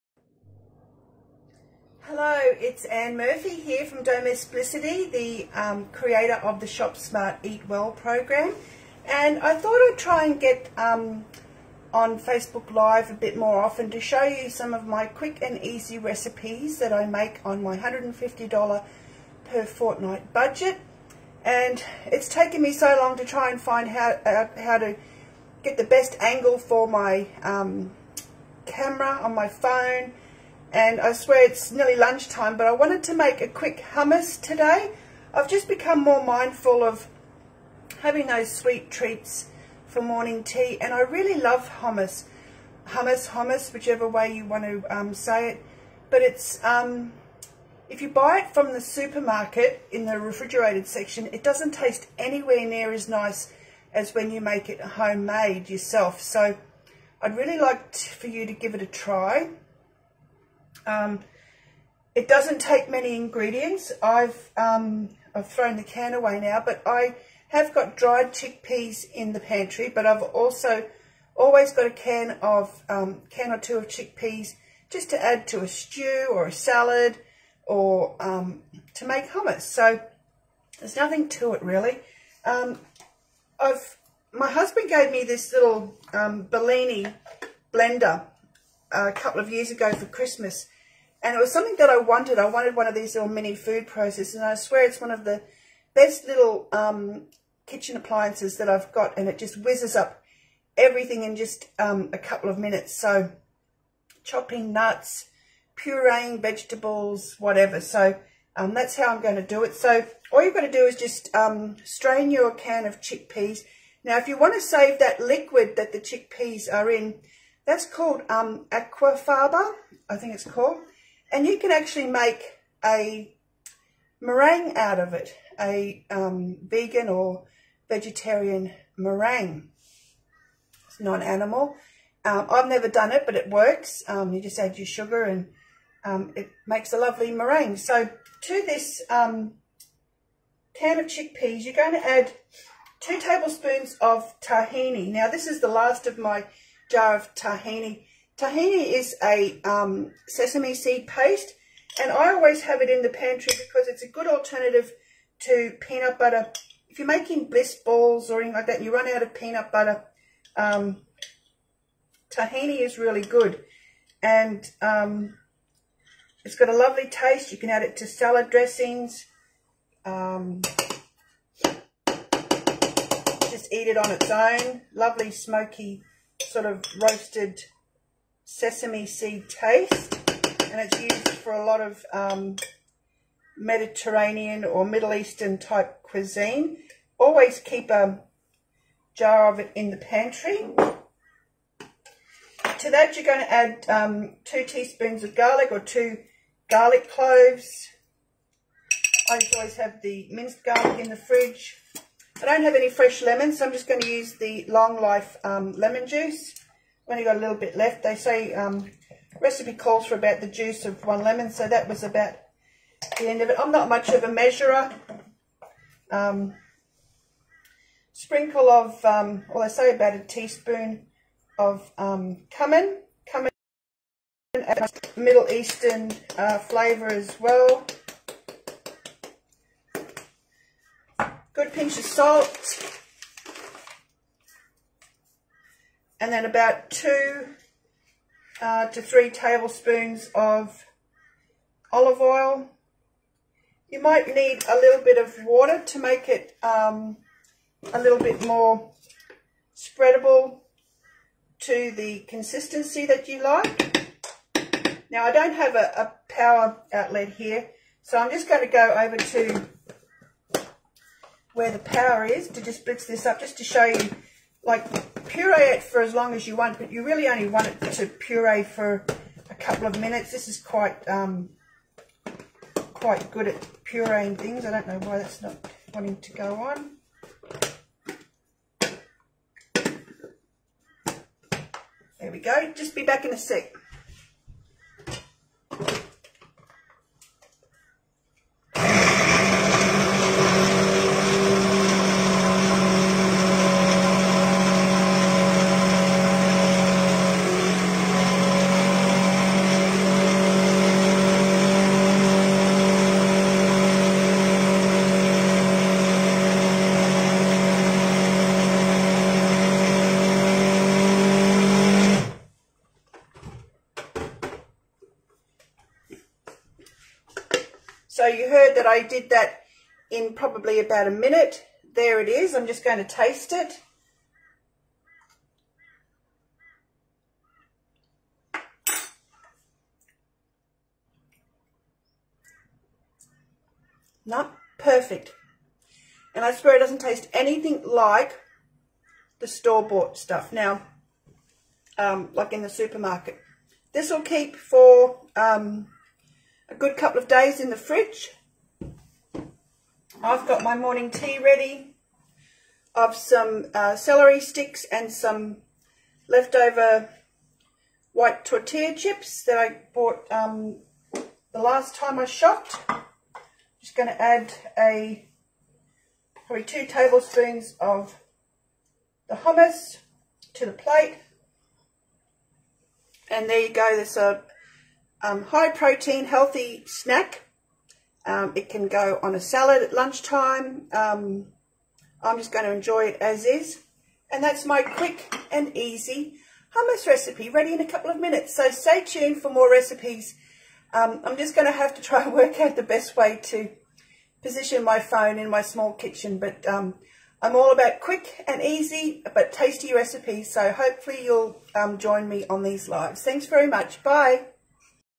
Quick & Easy Hummus Recipe in 5 Minutes
Take a can of chickpeas and add your basic pantry staples for this quick and easy hummus recipe. So much better than supermarket stuff.